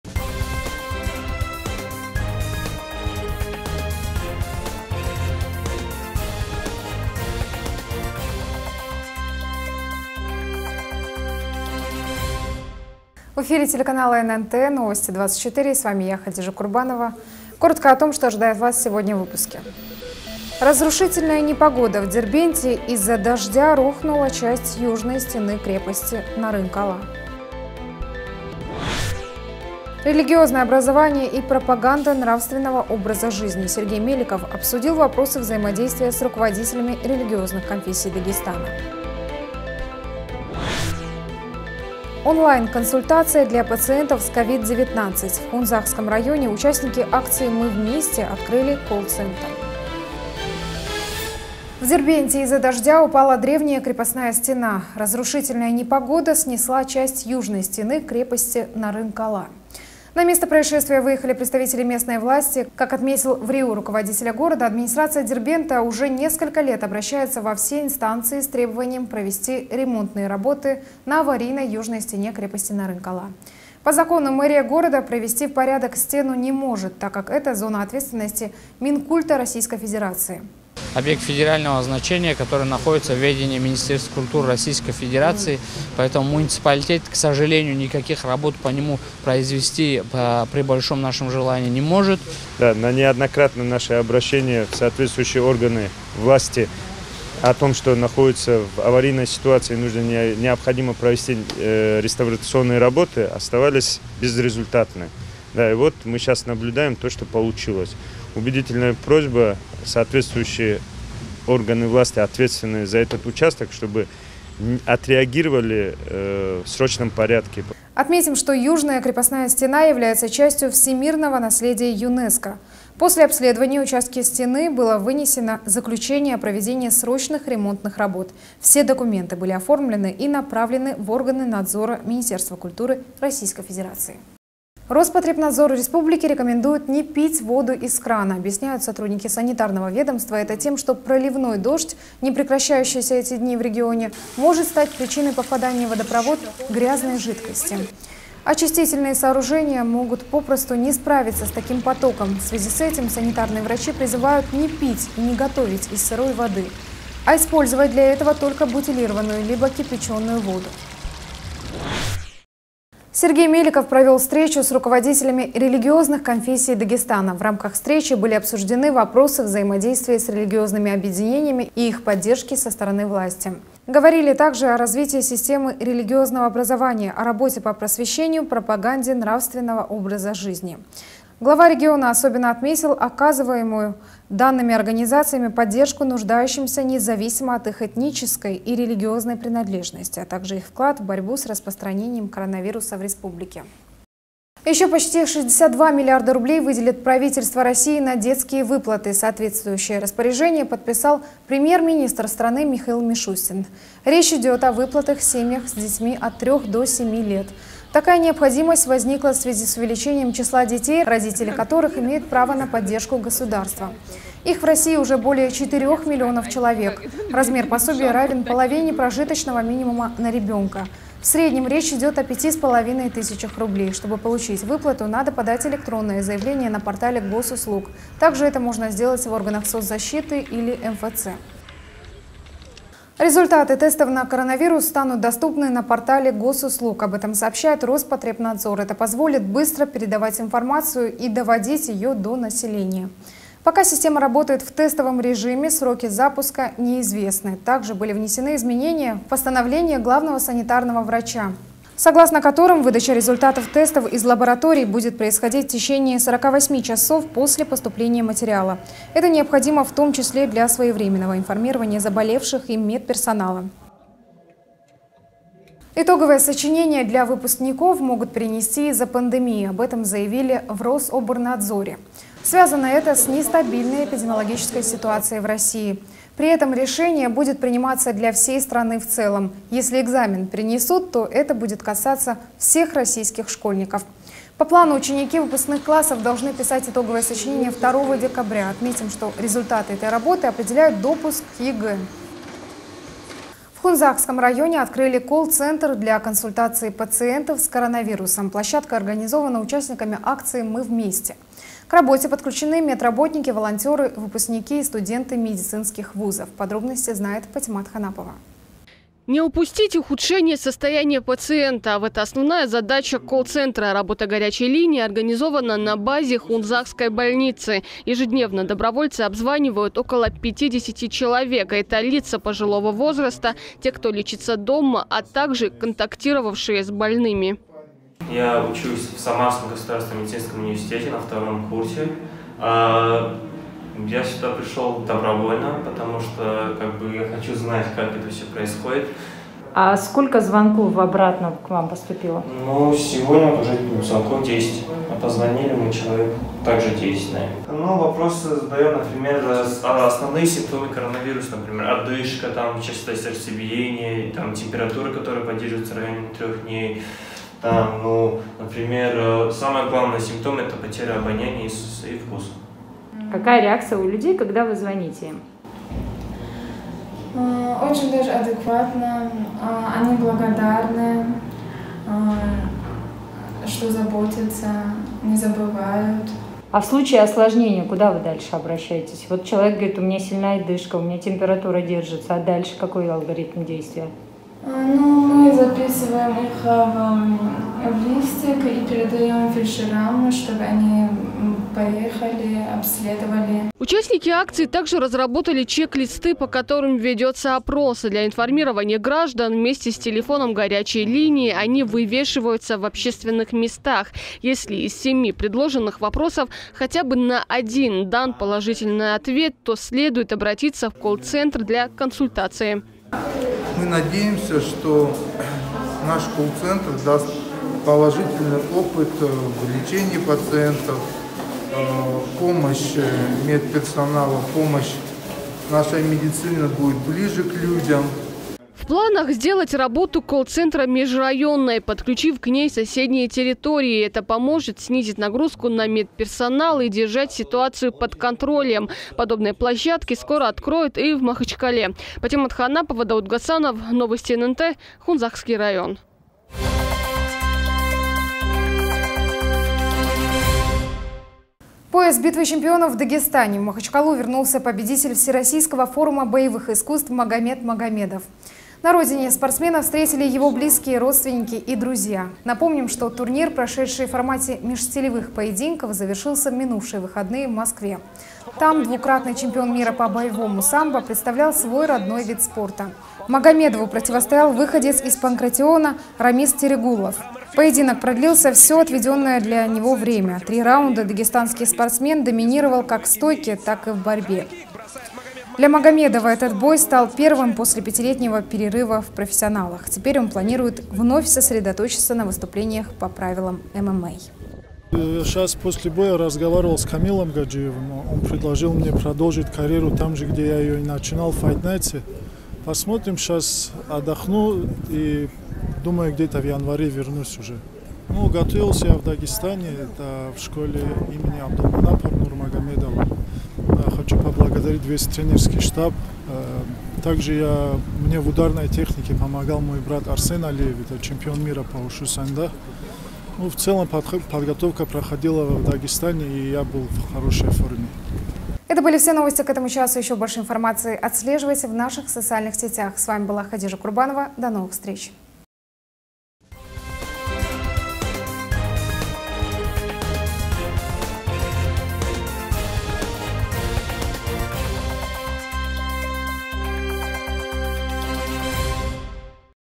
Новости Дагестана за 17.11.2020 года
1. Разрушительная непогода… В Дербенте из-за дождя рухнула часть Южной стены крепости Нарын-кала.

2. Религиозное образование и пропаганда нравственного образа жизни. Сергей Меликов обсудил вопросы взаимодействия с руководителями религиозных конфессий Дагестана.

3.Онлайн-консультация для пациентов с ковид-19. В Хунзахском районе участники акции «Мы вместе» открыли колл-центр.